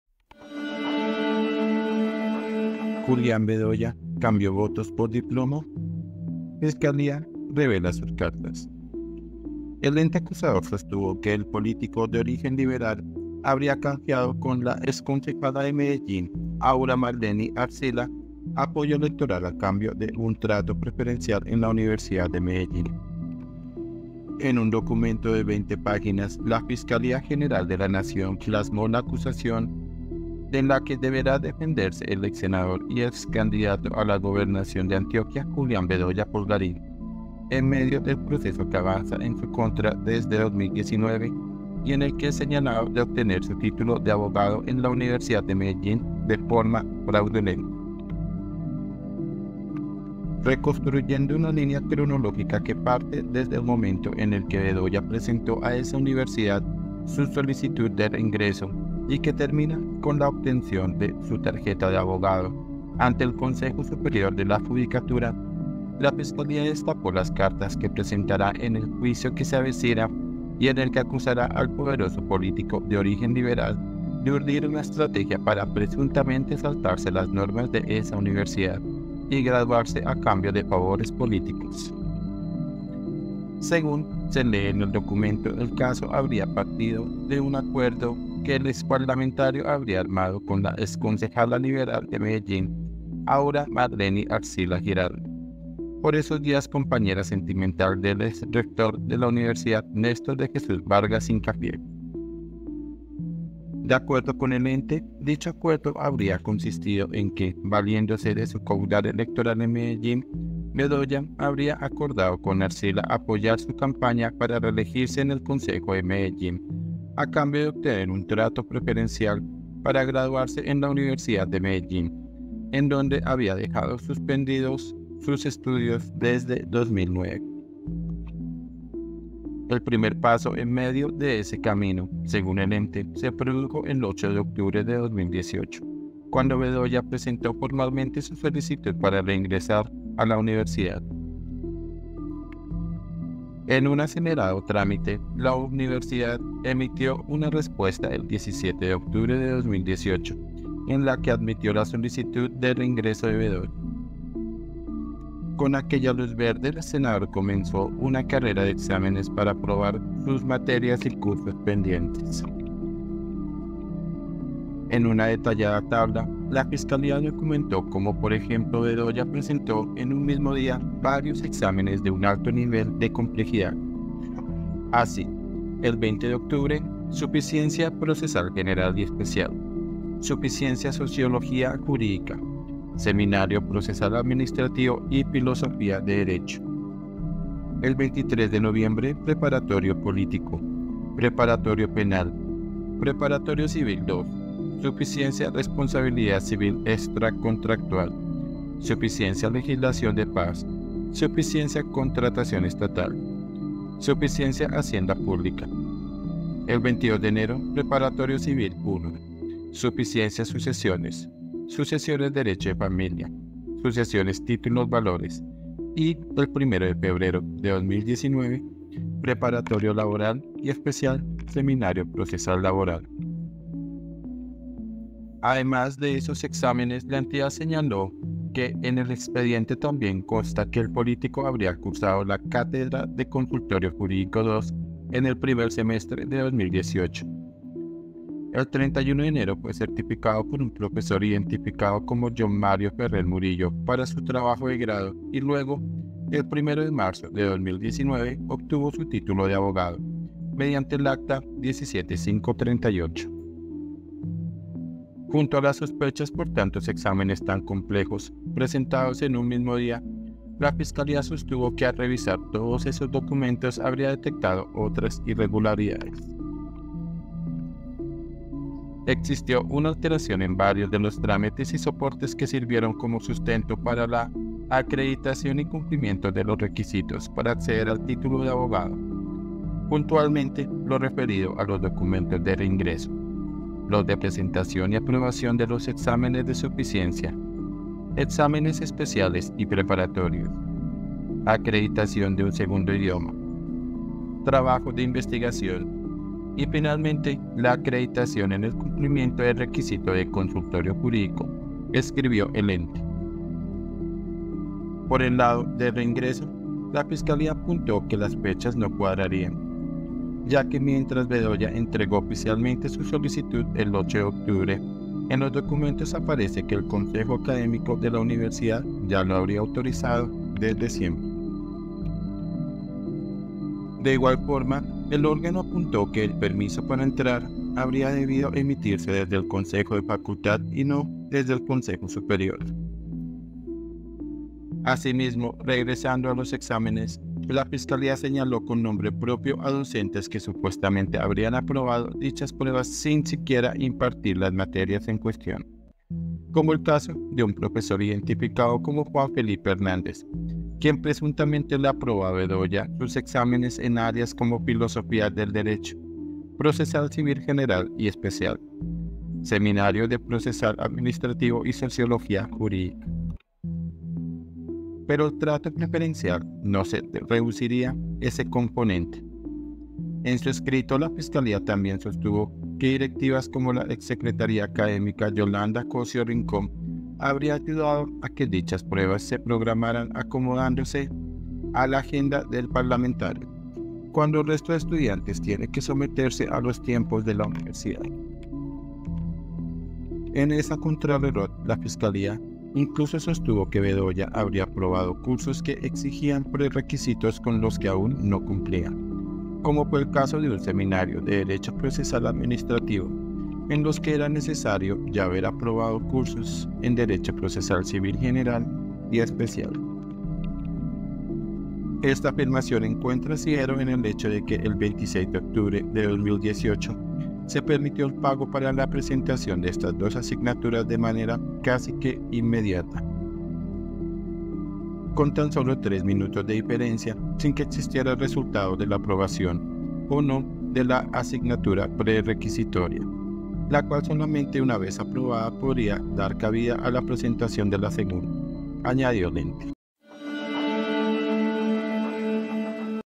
ESCÁNDALO 🔴 ¿JULIÁN BEDOYA CAMBIÓ VOTOS POR DIPLOMA?
El ente acusador sostuvo que el político de origen liberal habría canjeado con la exconcejala de Medellín, Aura Marleny Arcila, apoyo electoral a cambio de un trato preferencial en la Universidad de Medellín.
EL COLOMBIANO